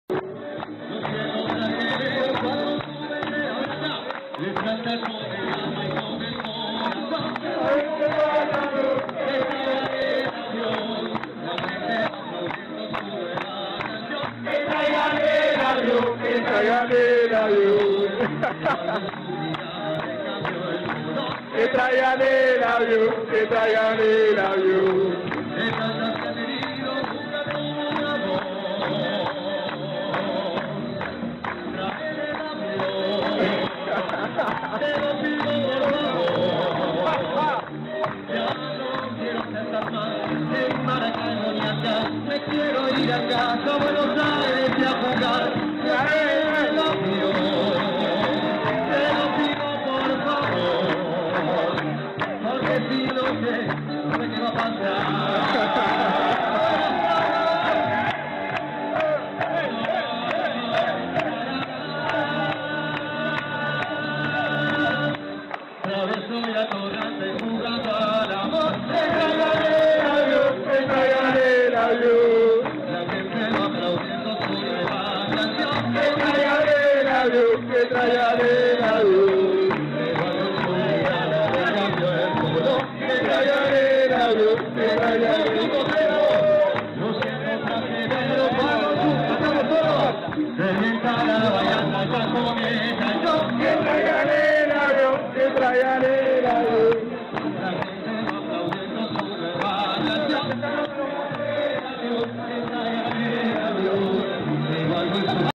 JUGADORES DE BOCA CANTANDO POR UN AVION
Diego Desanzo cantando en Venezuela con jugadores de Boca y Allboys